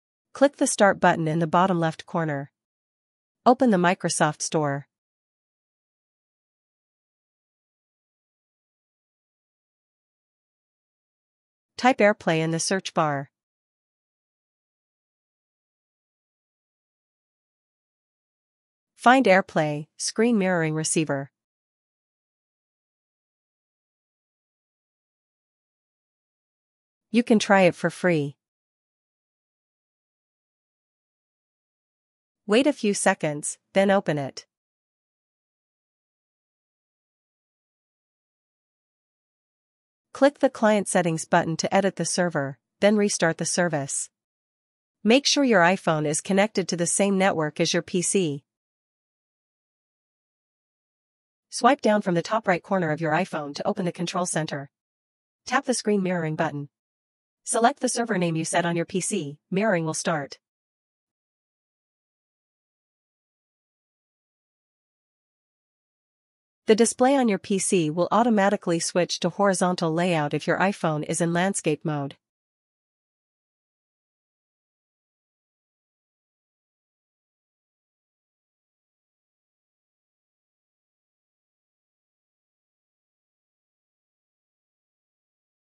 How to Use AirPlay Mirroring on Windows 11 – Mirror iPhone to PC
Mirror your iPhone or iPad screen to your Windows PC with the app: AirPlay - Screen Mirroring Receiver!

This video shows you:
✅ How to install the AirPlay - Screen Mirroring Receiver app
✅ How to connect your iPhone using AirPlay
✅ Live demo of screen mirroring in action

No cables, no setup — just open, connect, and enjoy your iPhone screen on your PC.

The app is built with UWP for fast, native performance on Windows 10 and 11.